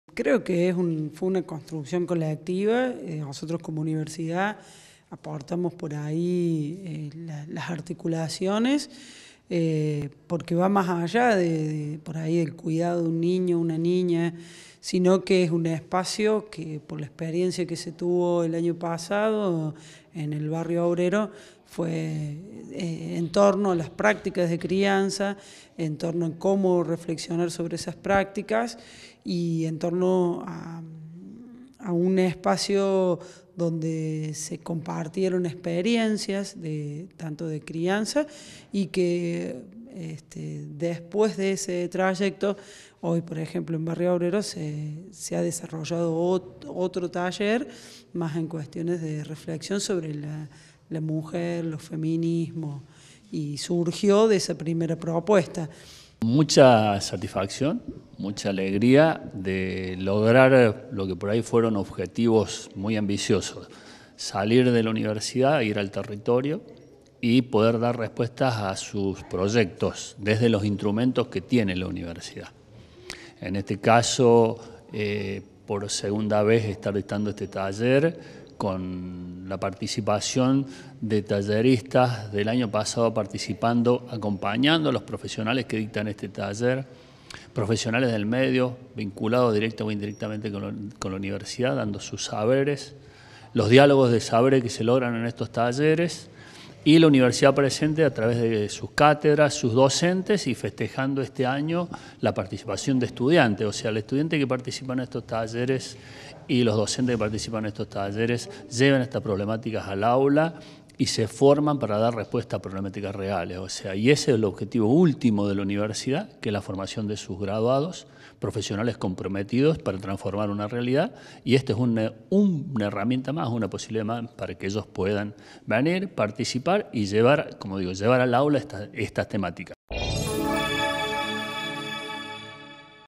CON UN ENFOQUE INTEGRAL, COMENZÓ TALLER SOBRE CUIDADO DE NIÑOS Y NIÑAS
ESTEFANÍA BRESSO - SUBCOORDINADORA CONSEJO SOCIAL UNRC
 MANUEL SCHNEIDER - COORDINADOR CONSEJO SOCIAL UNRC

Facebook.com/uniriotv
Twitter.com/uniriotv